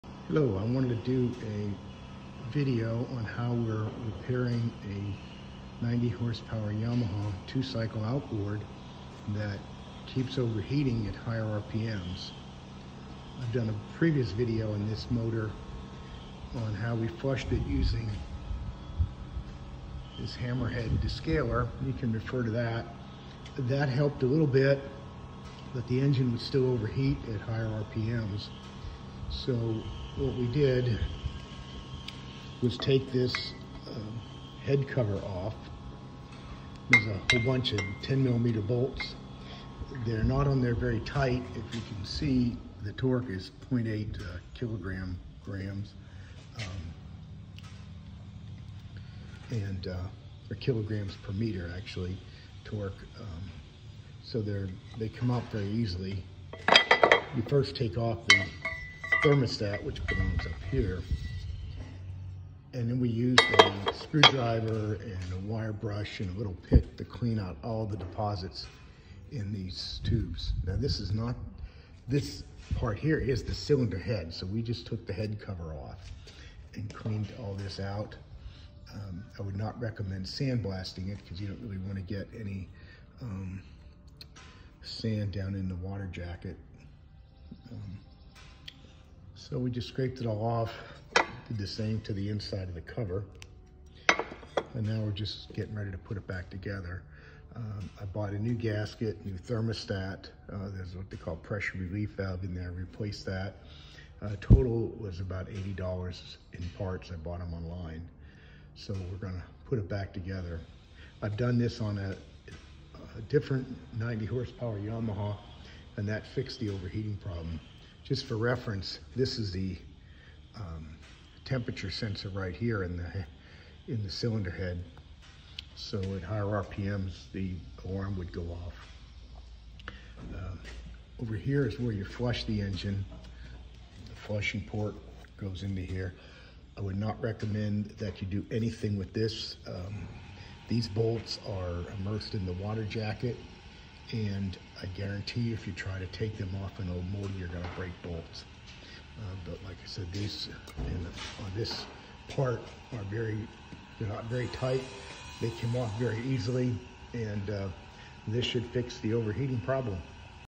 How to repair an overheating outboard
This video demonstrates how I removed the cylinder head cover in my 90hp yamaha 2 cycle outboard to clean the water jacket in the cylinder head to prevent overheating at higher RPMs.